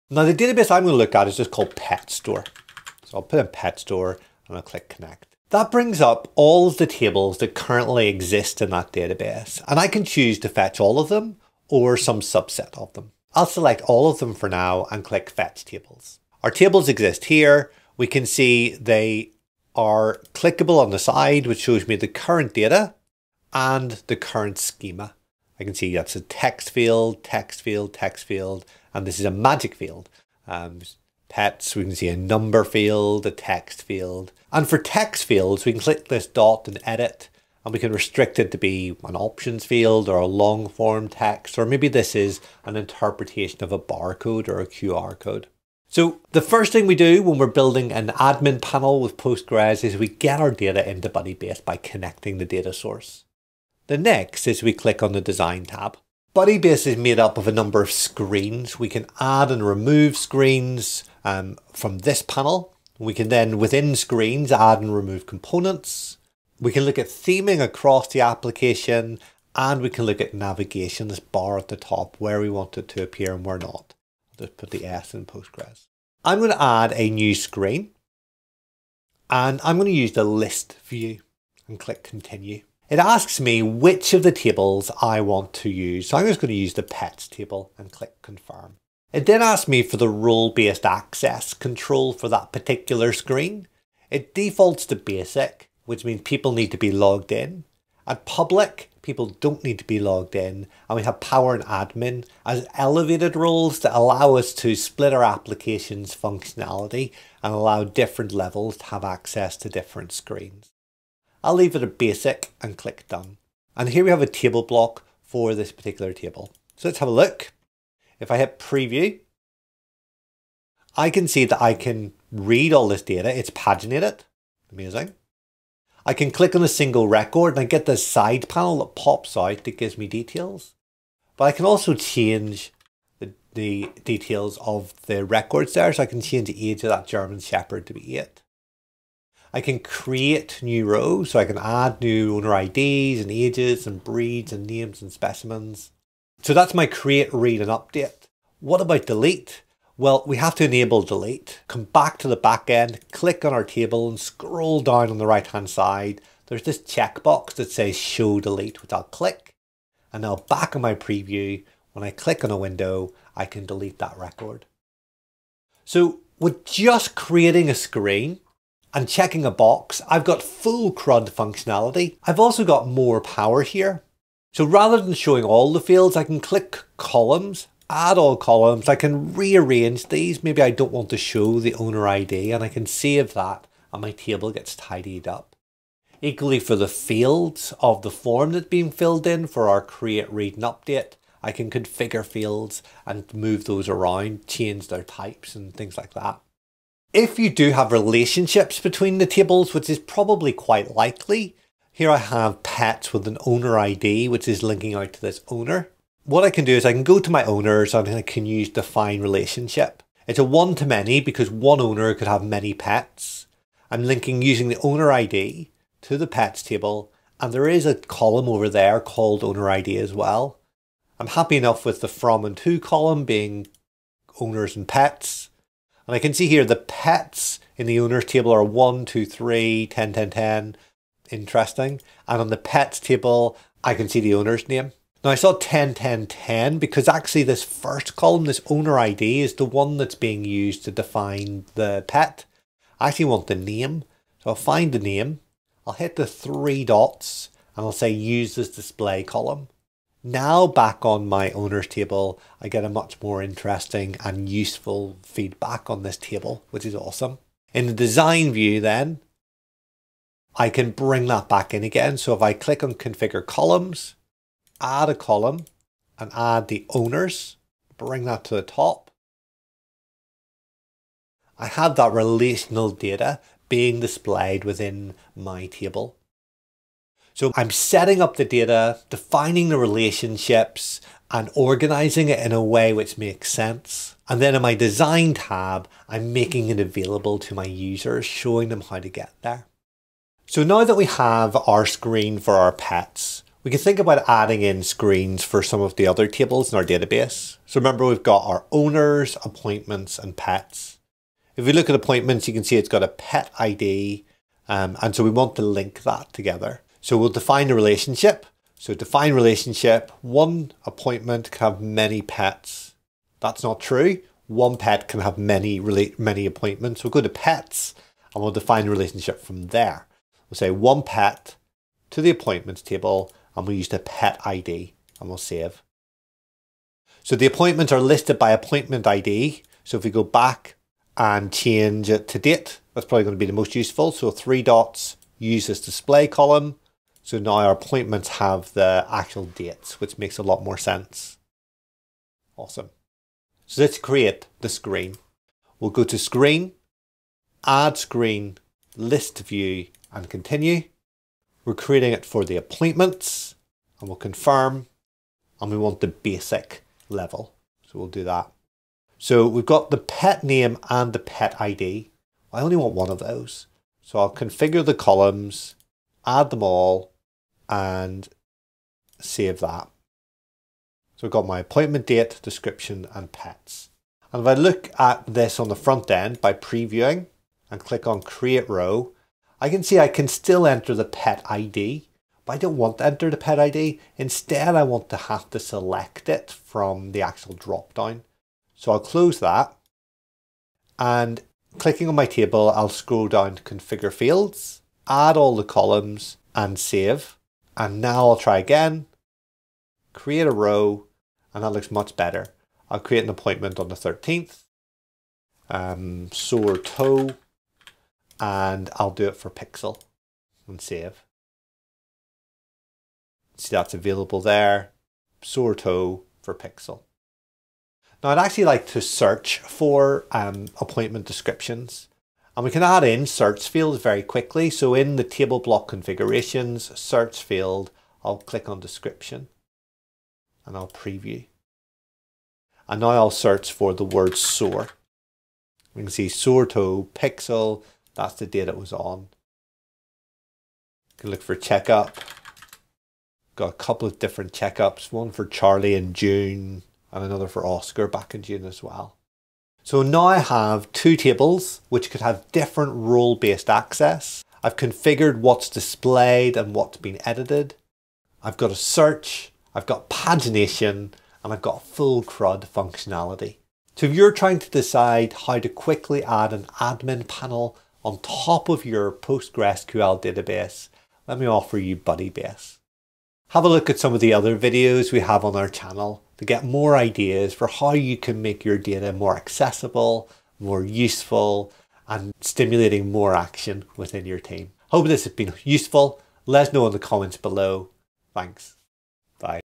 How to build a PostgreSQL admin panel from scratch and for FREE
In this video, I'm going to show you how to build a PostgreSQL admin panel from scratch and for FREE. This admin panel is perfect for managing your PostgreSQL database, and it's absolutely free!

This low code admin panel is perfect for anyone who wants to build a custom admin panel without having to spend hours coding. In this video, I'll show you how to create this admin panel in easy steps, and you'll even get downloads and installation instructions so you can start using it right away!